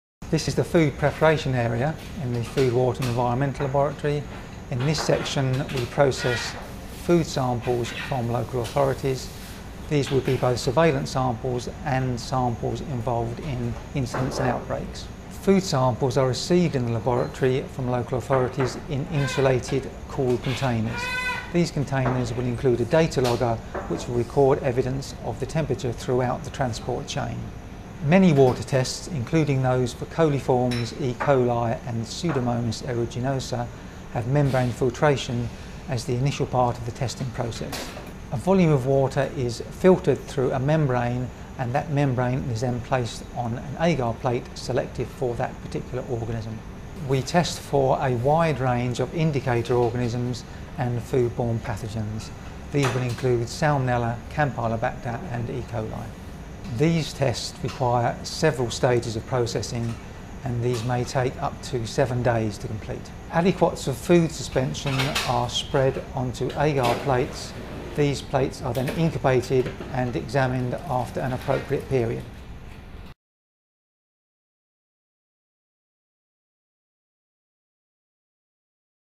Testing food & water for possible contamination
Martin Lodge from Public Health England, explains how food and water samples from local authorities are tested for possible contamination. Samples can be sent in from routine sampling or to investigate an outbreak of infection.